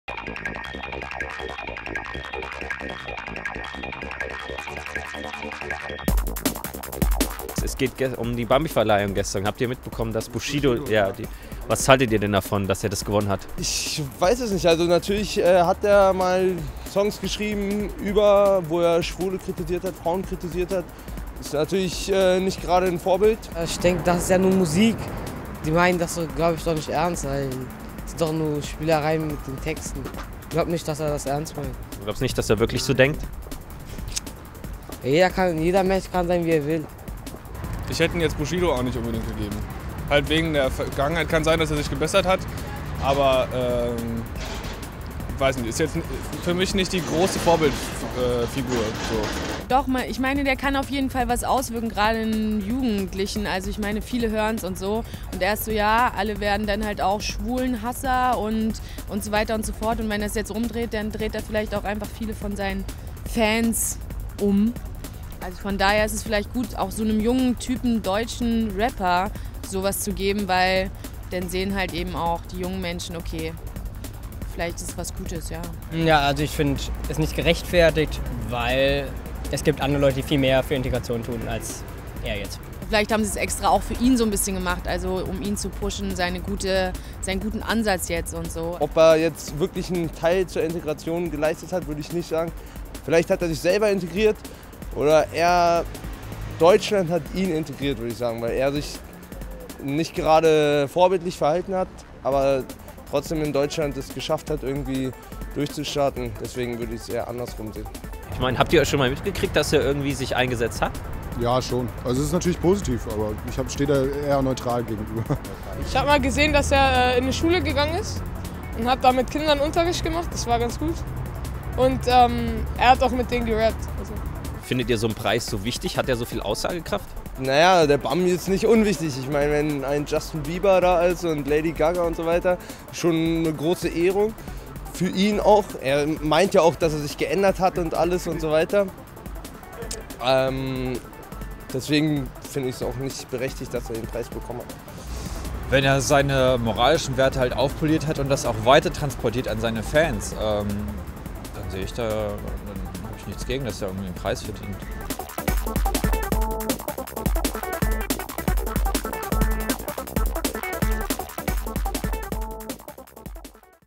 Was sagt die Straße: Bushido´s Integrations-Bambi
Gestern hat Bushido bei der Bambi Verleihung den Preis für Integration überreicht bekommen. Im Vorfeld, bei der Verleihung und nun auch im Nachhinein scheiden sich die Geister, ob er es verdient hat oder nicht.
Was hat Bushido für die Integration in Deutschland getan? Ist er ein gutes Vorbild? Disqualifizieren in seine teilweise schwulen- und frauenfeindliche Texte und Aussagen aus der Vergangenheit nicht für solch eine Auszeichnung?
Wir sind der Frage auf der Straße nachgegangen und wollten von den Menschen wissen, was sie zum Thema zu sagen haben.
Seht hier das Ergebnis!